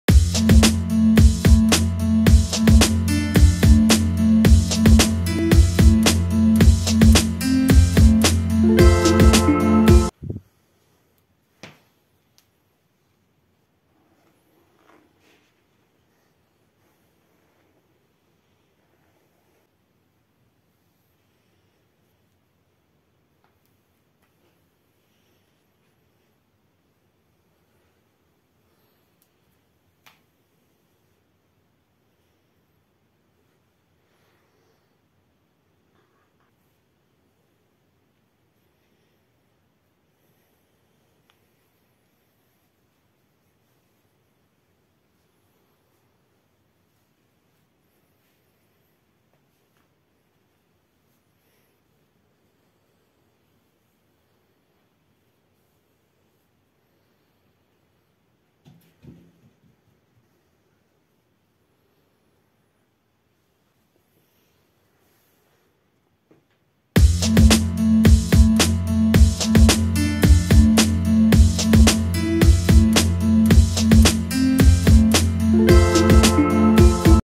Playing my hair: parting, brushing, pulling, braiding (trailer) (full video- link in comments)
In this video, you can see how my hair is first gently combed, and then pulled hard.  You will also see the parting of the hair, and how at the end they pull me by the braid)